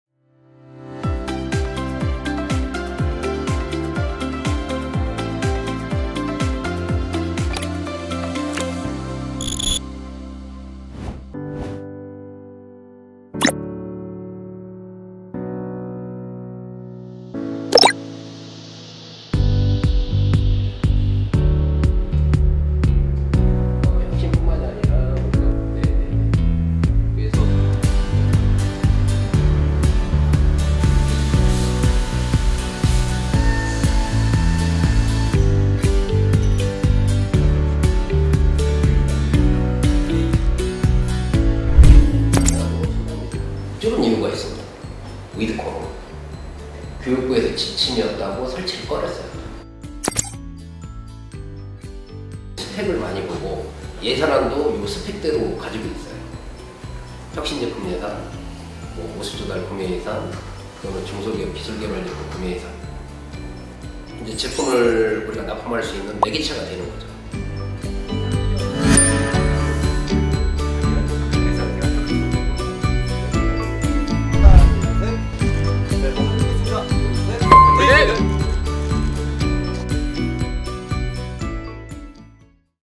[민생 현장을 가다 ① 전라북도 완주] 민생이 “최우선” 현장 속 생생한 목소리 듣다
11월 1일 김윤상 조달청장은 민생 현장 소통을 위해 전라북도 완주군에 있는 혁신‧우수조달물품 지정 기업인 에스엠엔테크를 찾았습니다.😄

에스엠엔테크의 ‘AIRJOY 공기청정살균기’
 - 2020년 조달청 혁신 시제품 지정된 이후 시범구매사업에서 ‘성공’ 판정
 - 우수조달물품 신청 자격을 획득
 - 2023년 제1회 우수조달물품으로 지정

이번 방문은 민생 현장 속 소통을 위한 것으로 중소‧벤처기업이 현장에서 겪는 어려움을 듣고
조달정책에 반영하는 동시에 판로 지원 방안을 모색하기 위해 마련했습니다.

민생 현장 방문은 전라북도를 시작으로 전국 곳곳의 조달기업으로 이어집니다.
앞으로도 조달청의 '민생 현장을 가다' 기대해 주세요😃👍